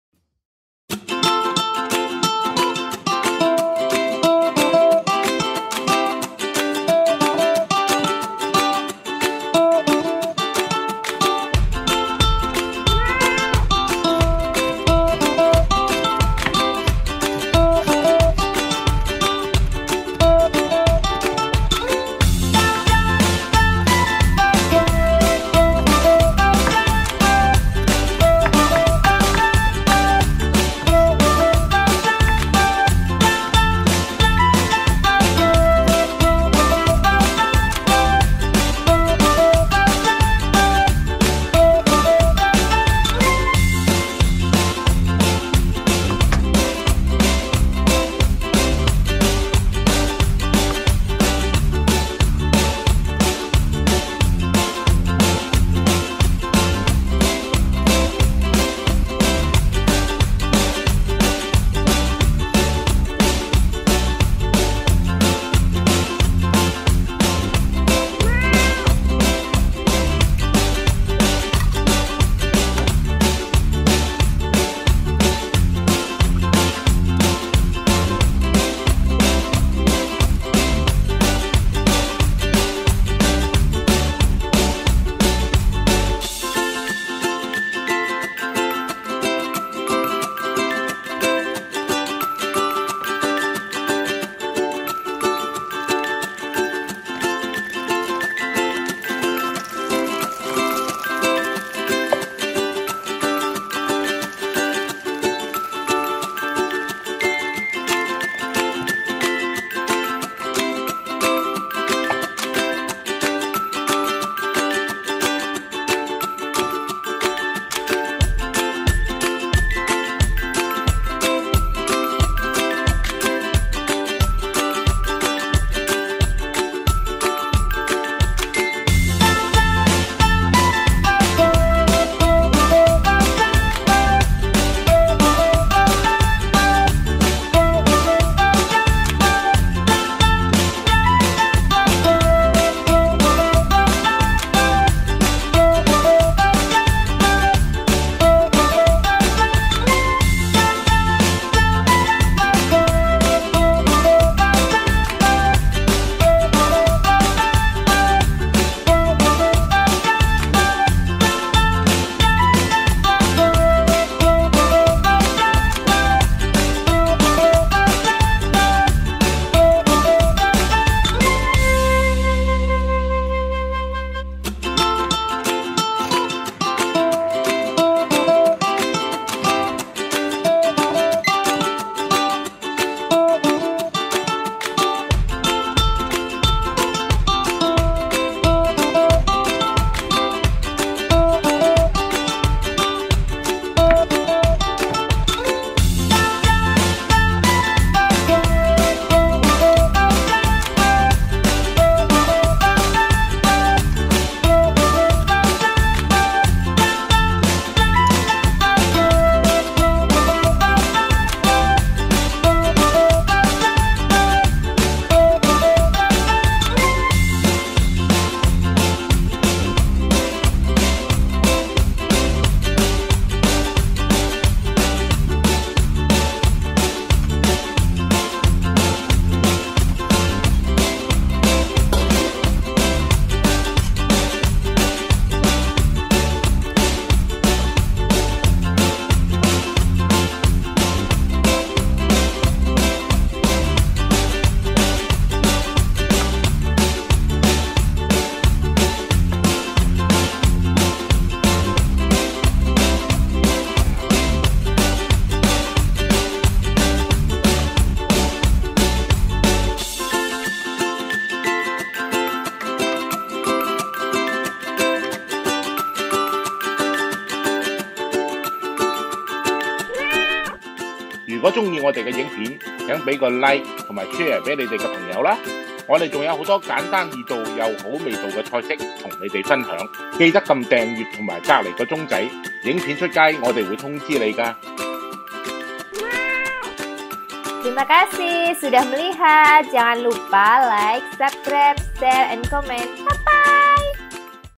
【BeluBaba】泰式蝦蛋卷🦐🥚原來蝦滑和蛋可以這樣食😳Shrimp Egg Roll with Thai Dipping Sauce【ENG Sub】
【我教我家工人姐姐做大廚】

今日來跟大家分享這個突如其來的想到的菜式：泰式蝦蛋卷～普通的一個煎蛋，加了蝦滑，豐富了口感和味道，而且這個菜十分軟腍，老少都很合適！最後再點上泰式魚露醬汁，立刻以為去了泰國😂😂

材料Ingredients：
－蝦滑Minced Shrimp
－雞蛋 Egg
－芫荽Coriander
－蔥Green onion